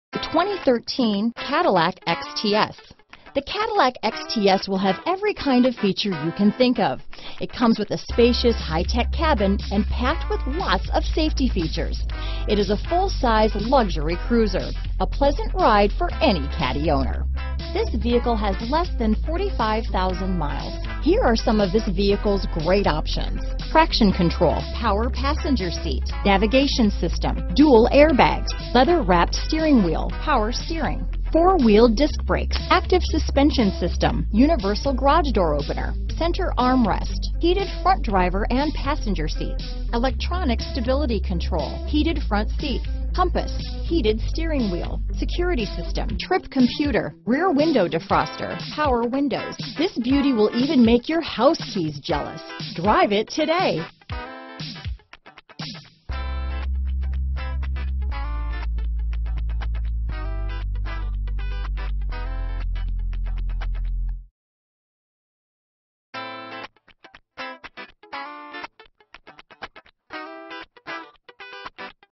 2013 Cadillac XTS Hampton, Chesapeake, Williamsburg, Yorktown, Newport News, VA 268190A
White Diamond Tricoat U 2013 Cadillac XTS available in Newport News, Virginia at Suttle Motor Corporation. Servicing the Hampton, Chesapeake, Williamsburg, Yorktown, VA area.